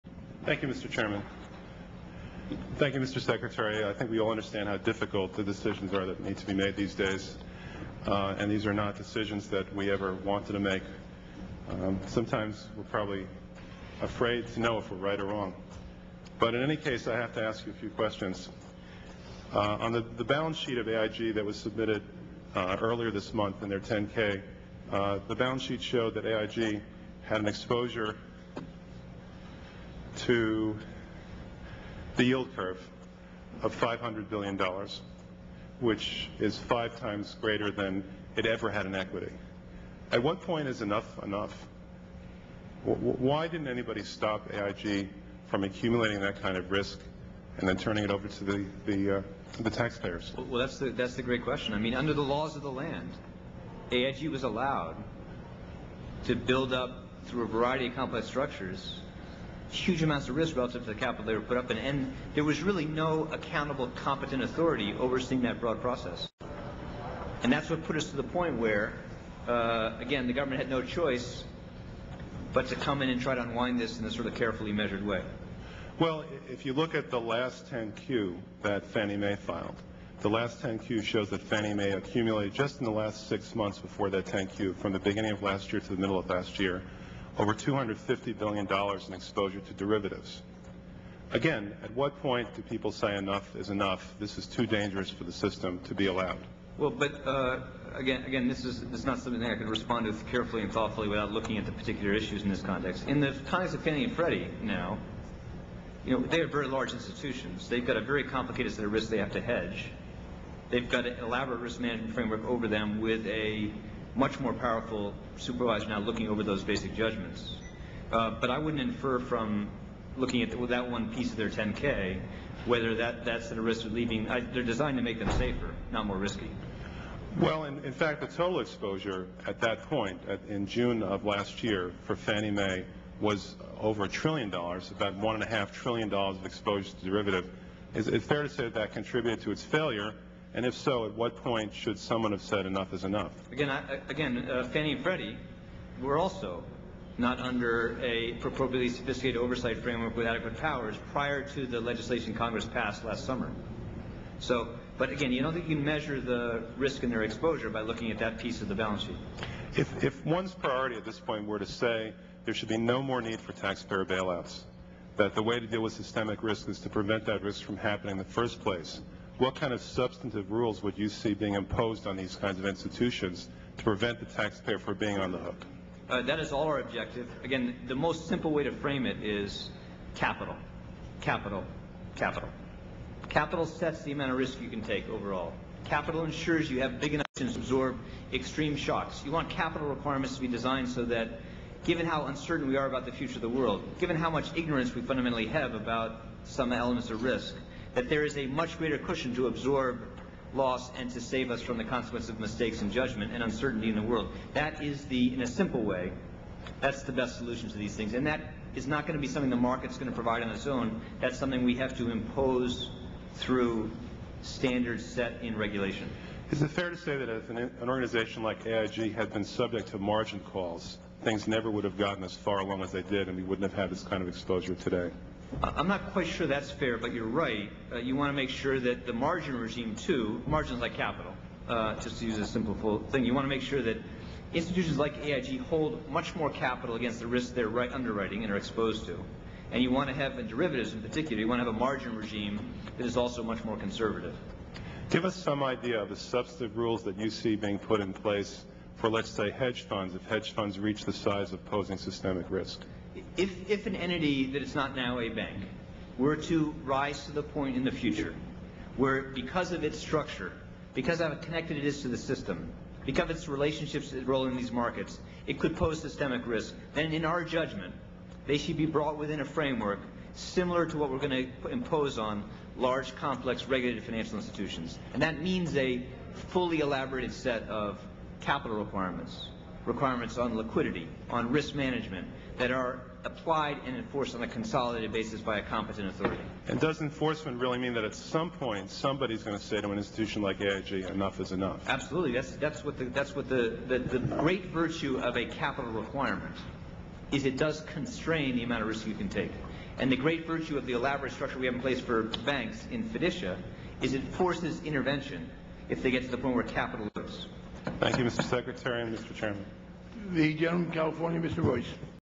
Grayson to Geithner: What Rules Do We Need to Prevent the Taxpayer from Being on the Hook?
Congressman Grayson and Secretary of the Treasury Tim Geithner discuss the AIG and Frannie/Freddie situations.  Grayson's questions are organized around the theme of what kinds of proscriptive rules need to be in place to preventing further taxpayer bailouts of the financial sector.  Geithner largely focuses on the need for capital requirements, liquidity requirements, and risk management applied and enforced by a competent authority.